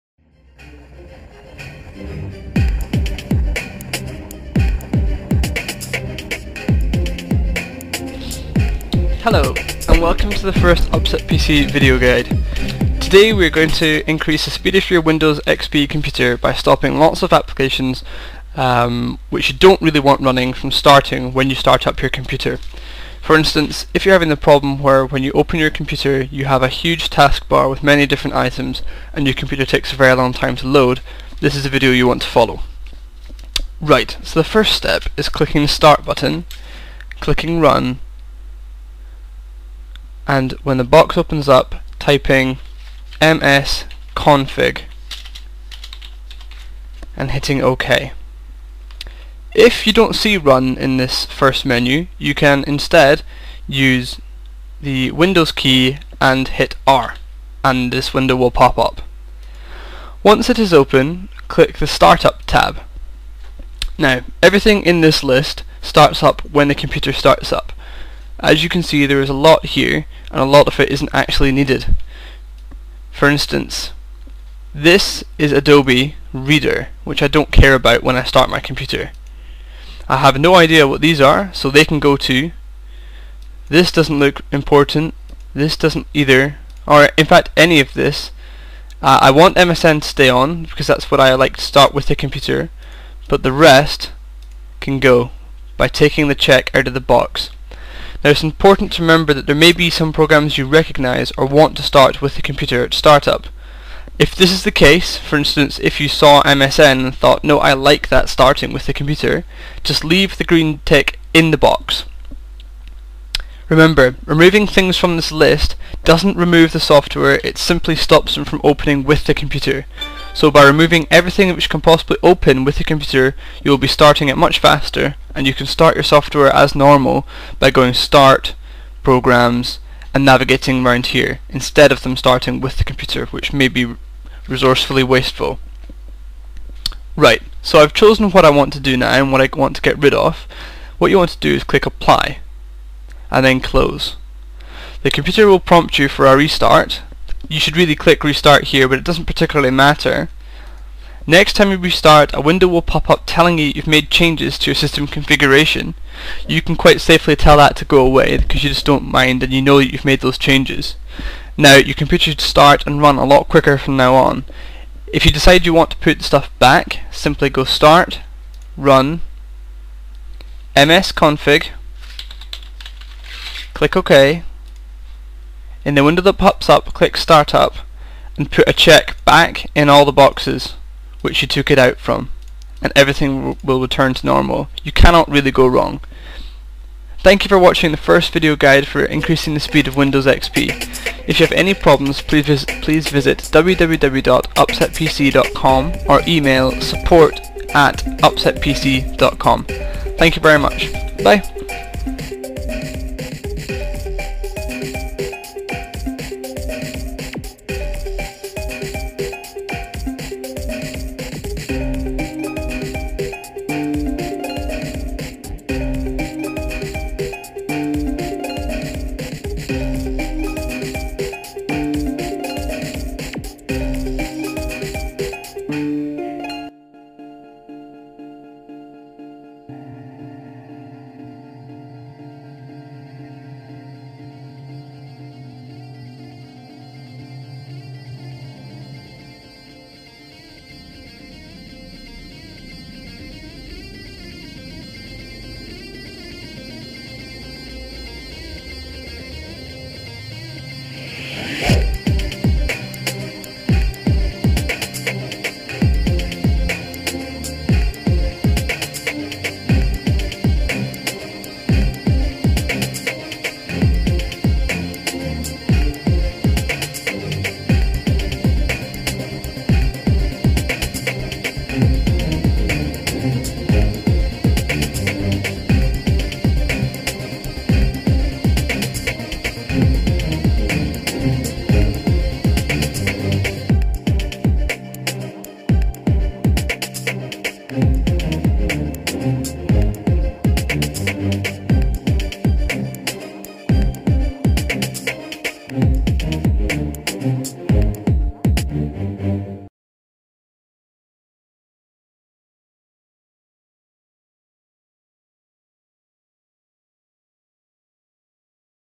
Windows xp speed up guide
This is an introduction video from UpsetPC.com explaining how to speed up windows xp's startup time and general running speed. It teaches the basics of using the msconfig menu to disable unneeded startup programs and gives tips on how to increase the speed of windows XP. This is a perfect place to start your windows xp tuning!